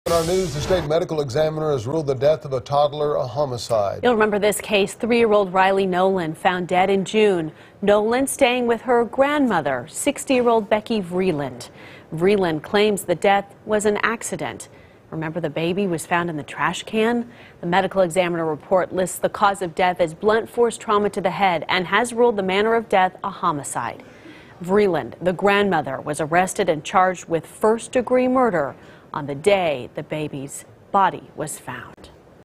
Medical examiner rules toddler's death a homicide
The child died from blunt force trauma to the head